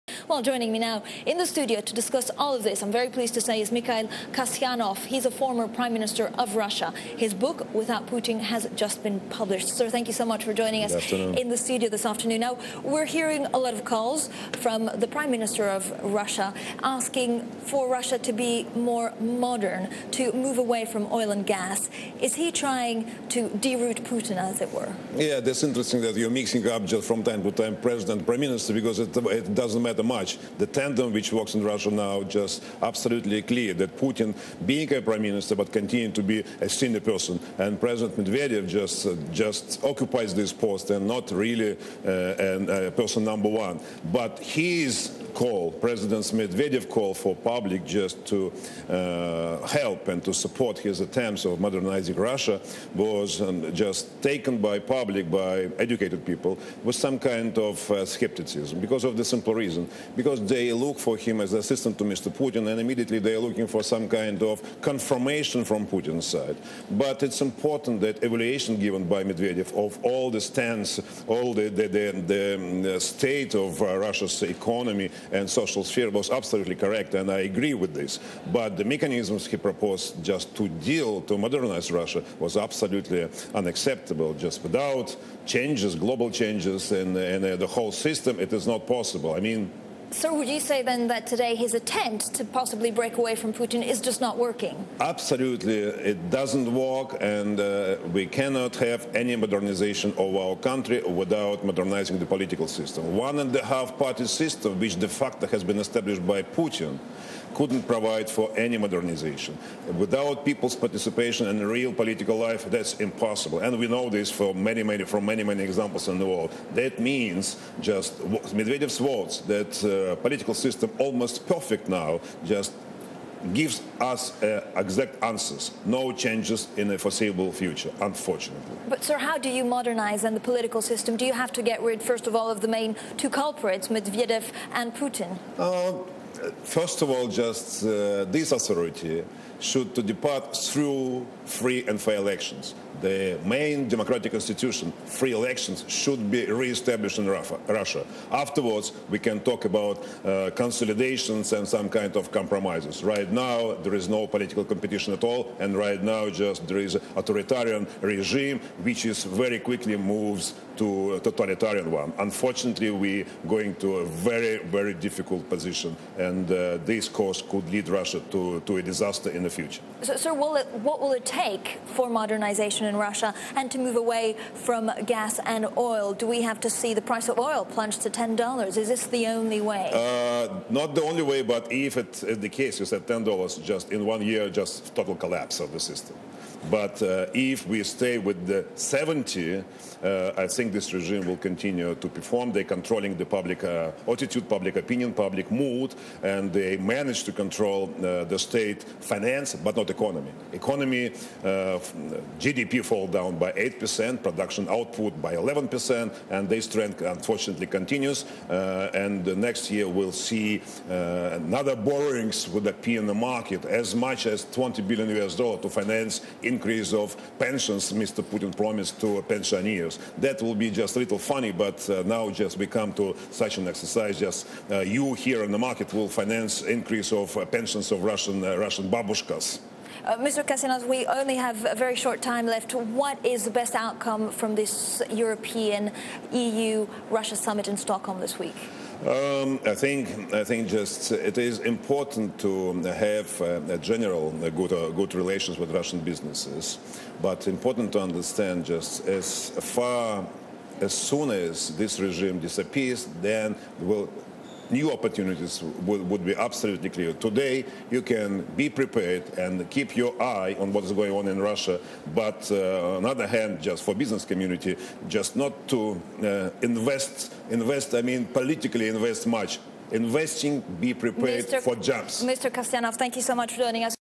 Kasyanov Says Russia Needs Free Elections Reestablished: Video
Nov. 17 (Bloomberg) -- Former Russian Prime Minister Mikhail Kasyanov talks with Bloombergs Francine Lacqua about the outlook for free elections in that country.
     Kasyanov, author of the book, "Without Putin," also discusses the implications of Russia's reliance on oil and gas revenue, and the upcoming summit meeting with the European Union. (Source: Bloomberg)